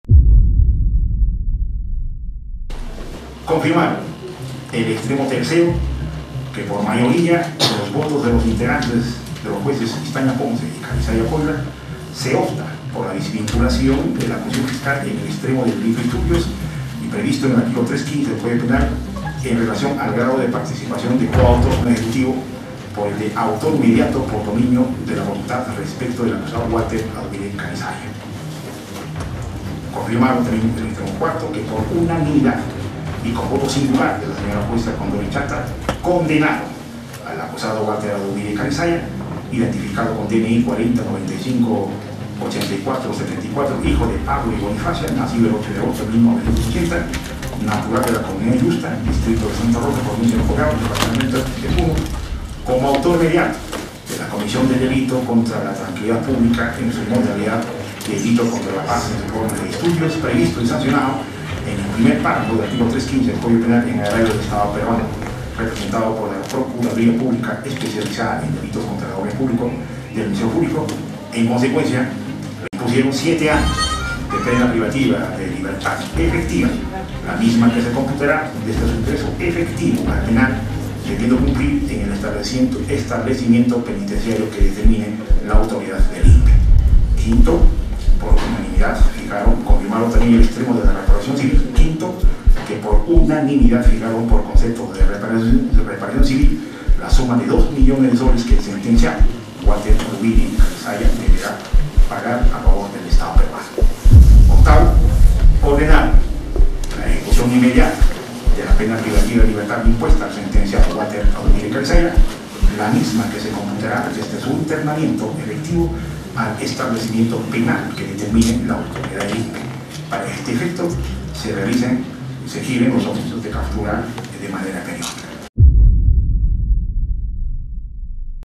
SENTENCIAN A 7 AÑOS DE PENA PRIVATIVA DE LIBERTAD A WALTER ADUVIRI
La Sala de Apelaciones de la Corte Superior de Justicia de Puno, ha decidido confirmar la sentencia del caso "aymarazo", ordenando se disponga la captura de Walter Aduviri Calisaya acusado por el delito de disturbios.

La Sala ha determinado que sí hubo delito, además le dio la razón al Juzgado Penal Colegiado, respecto a la desvinculación en relación a su participación como autor mediato.

Walter Aduviri tiene una sentencia condenatoria a 7 años de pena privativa de libertad y al pago de una reparación civil de dos millones de soles
fuente: recopilación mixta de vídeos

 PAGINAS EXTERNAS:
CHOLO SOY:
REGIONAL MC.